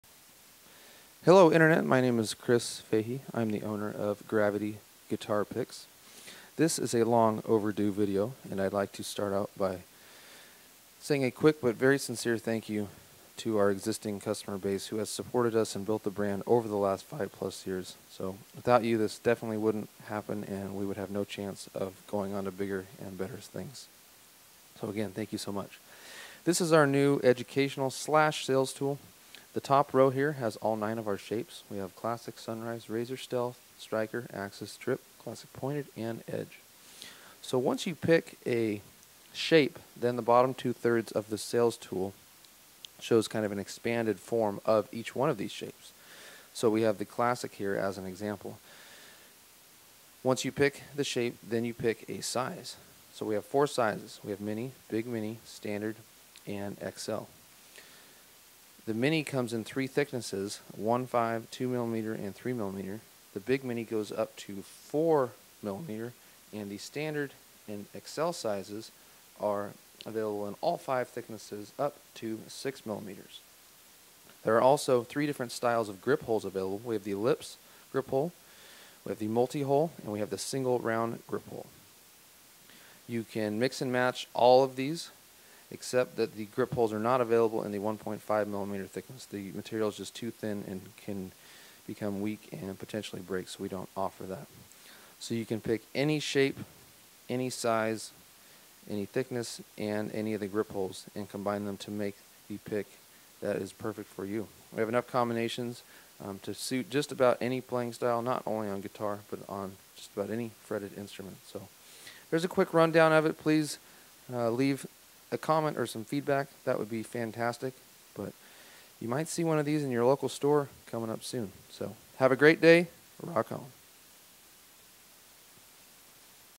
Gravity Picks Educational Video
This is our new sales / educational wood display board.  It shows the 9 shapes and the expanded view of one of the shapes.